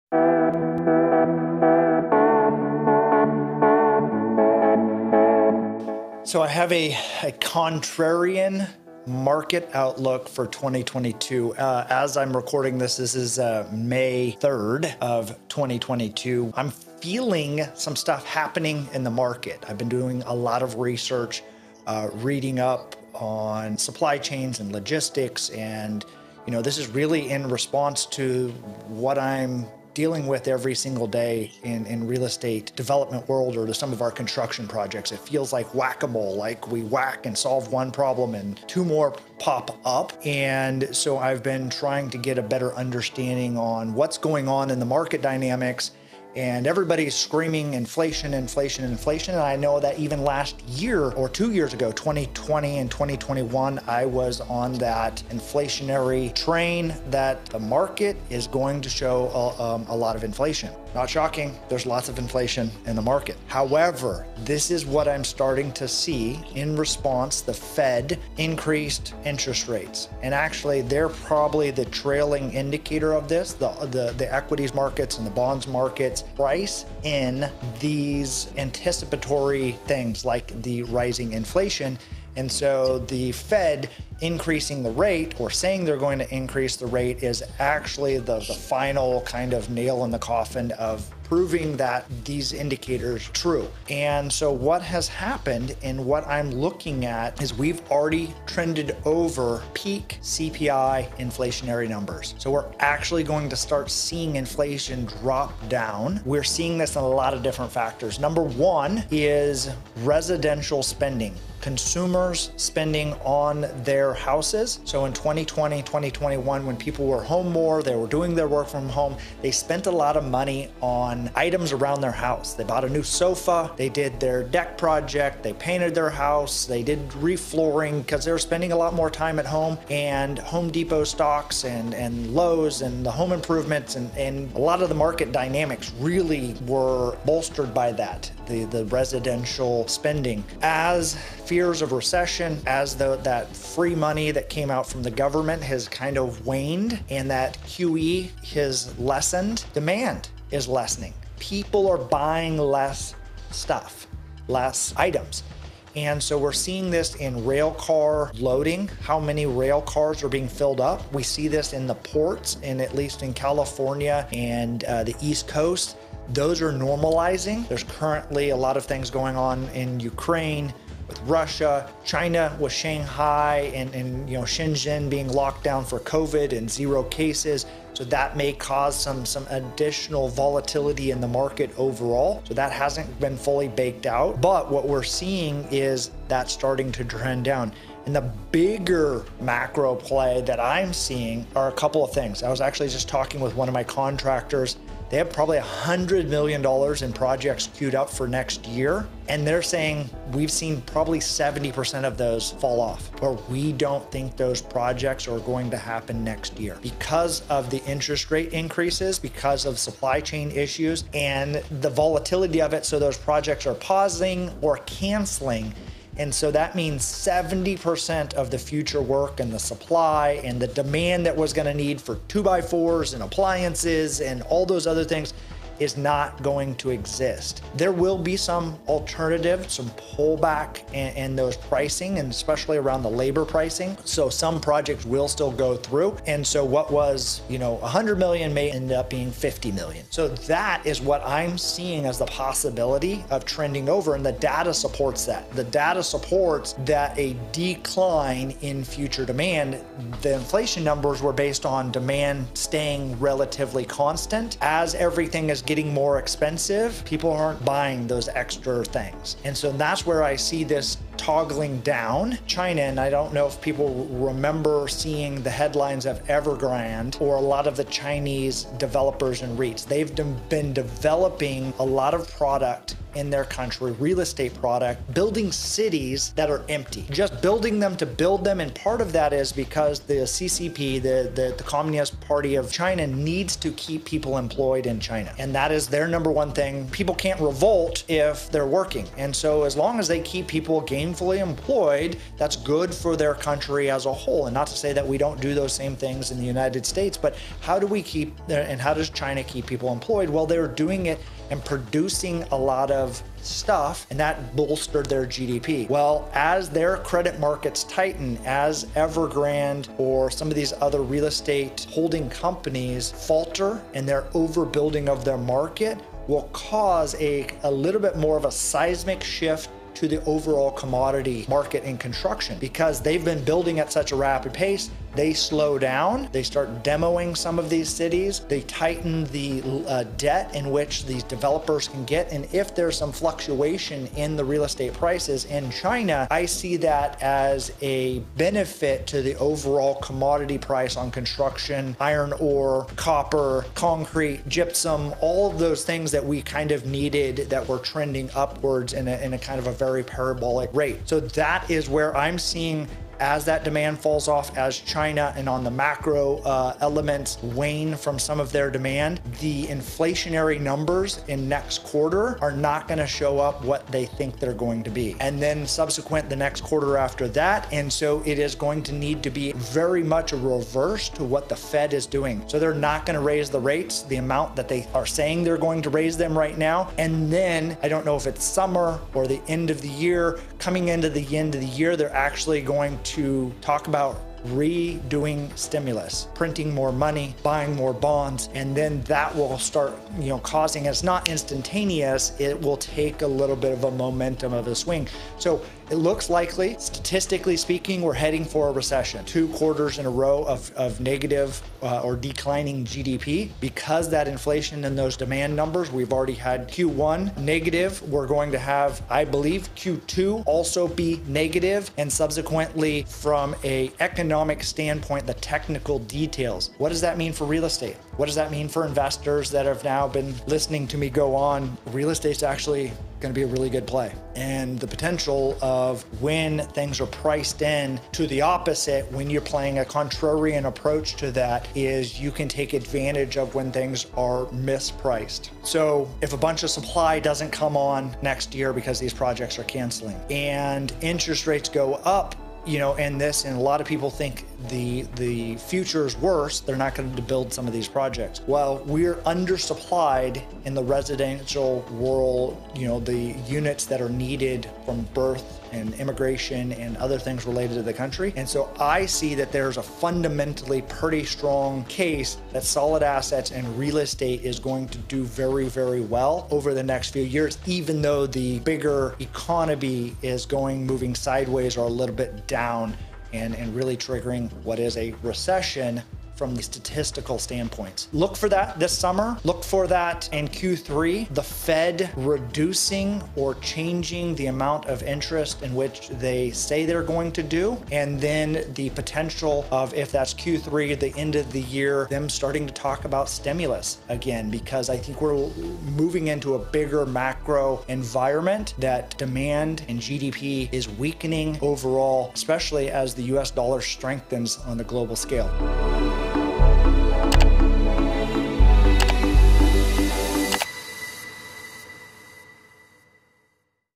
How to calculate risk in a market with inflation in 2022
Looking for ways to calculate risk in the current market we are in? Inflation and fears of a recession have heavily influenced the market and where it is heading. In this video, I share more about inflation, recession, and the market so you can make better decisions and calculate risks in your investment.